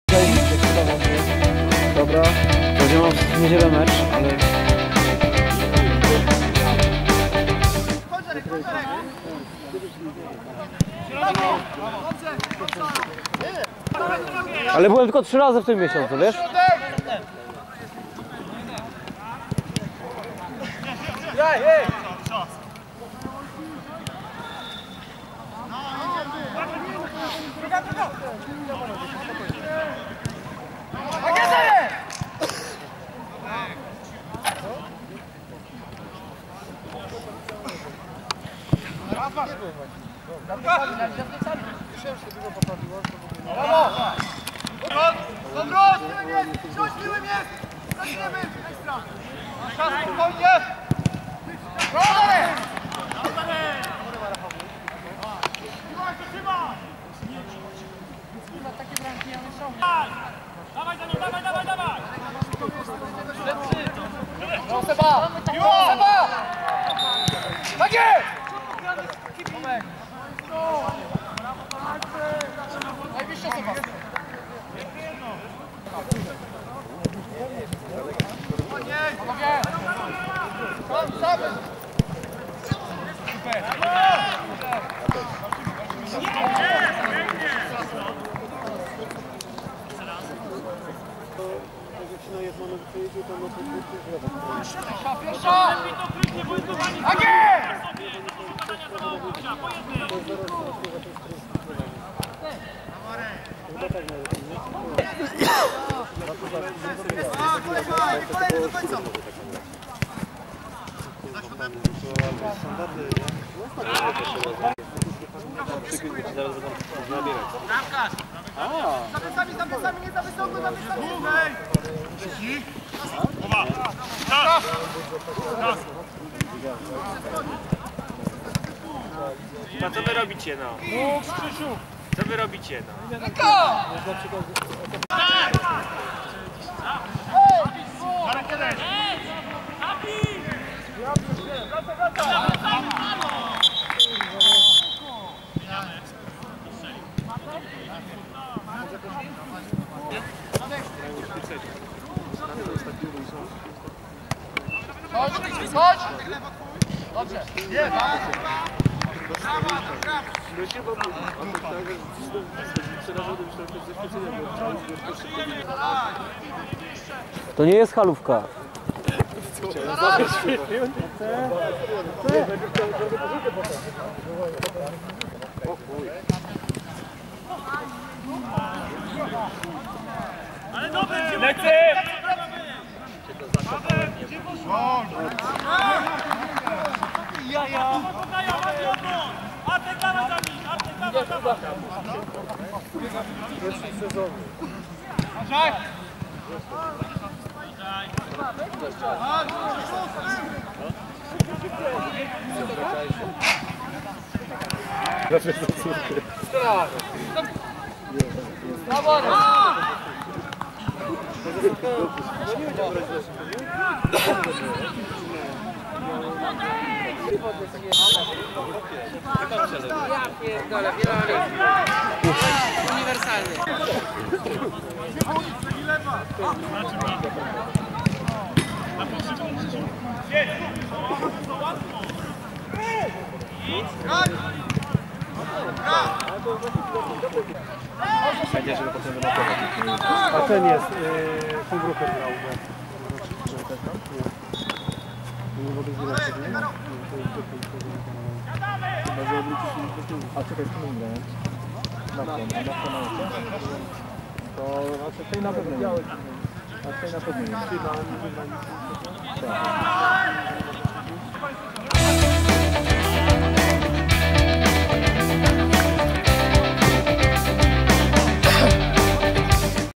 Pizzeria Football - Format: 3. tydzień (FLS Wiosna 2017)
Skrót meczu: Pizzeria Football - Format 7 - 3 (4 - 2)